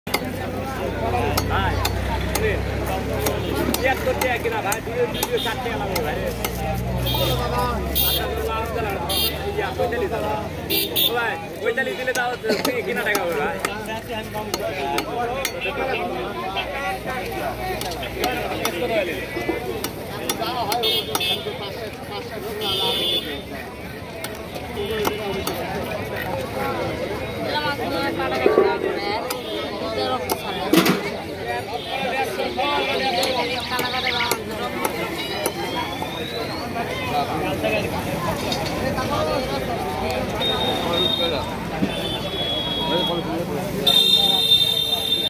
Best Fish Cutting Video | Amazing fish cutting skills
Watch amazing  fish cutting and chopping skills by an expert fish cutter at Bangladesh fish market.
 Enjoy professional fish cutting techniques by the fish cutting expert.
 I hope, you will enjoy this fish cutting video.

*** ANTI-PIRACY WARNING ***
 redistribution or re-uploading in any media is strictly prohibited of this material.